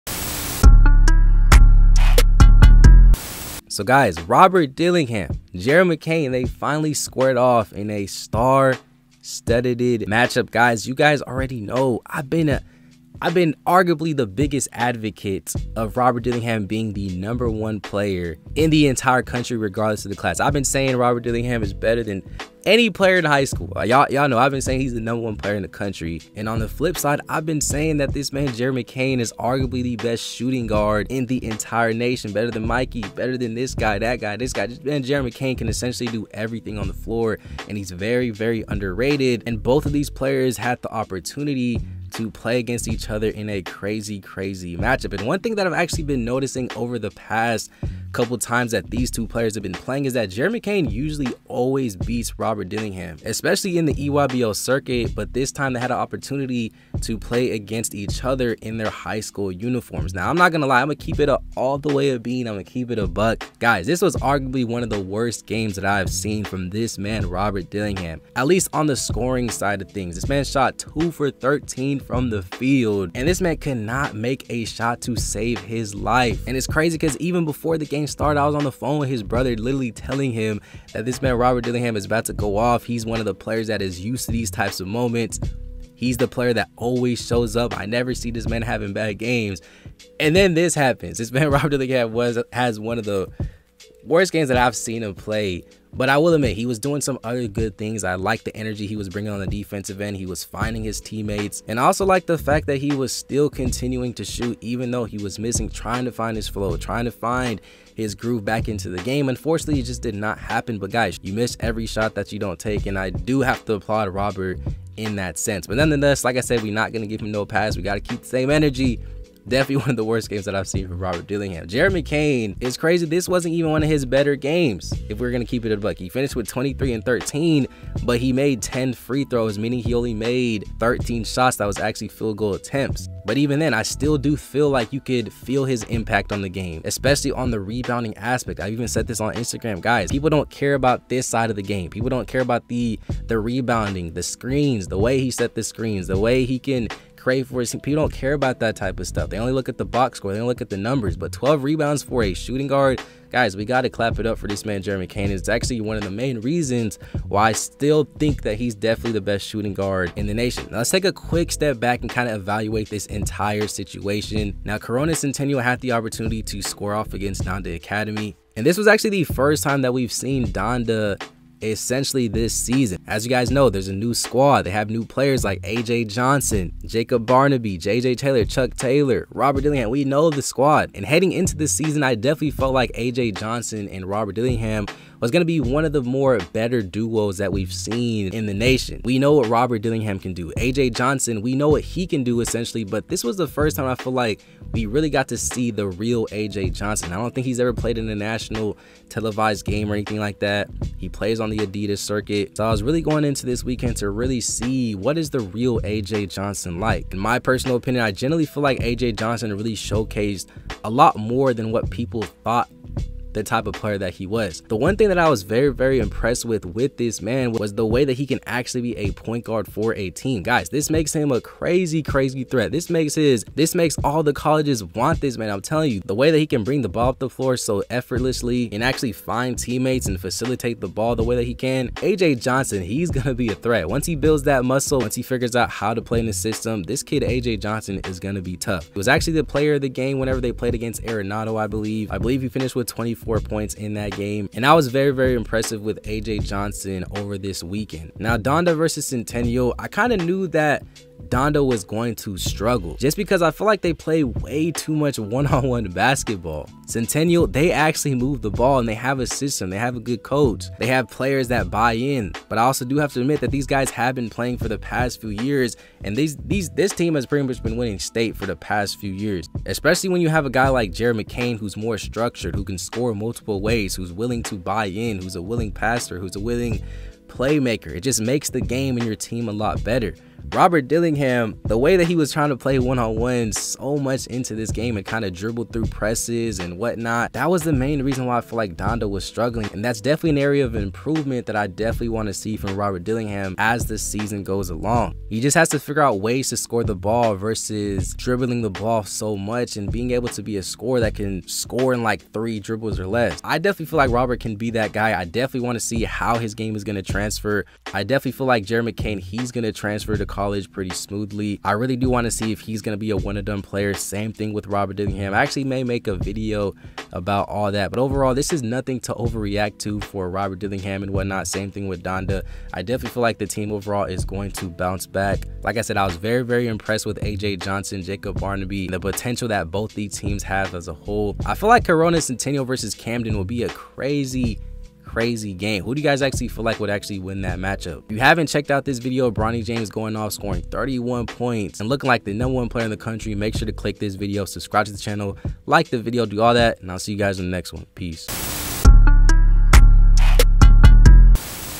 WOW... JARED MCCAIN EXPOSES ROB DILLINGHAM & DONDA ACADEMY...
WOW... JARED MCCAIN JUST EXPOSED ROB DILLINGHAM & DONDA ACADEMY...

Donda Academy and Corona Centennial squared off in an epic matchup and in this video I breakdown Rob vs Jared.

NONE Of The Clips Used In This Video Are Owned By Me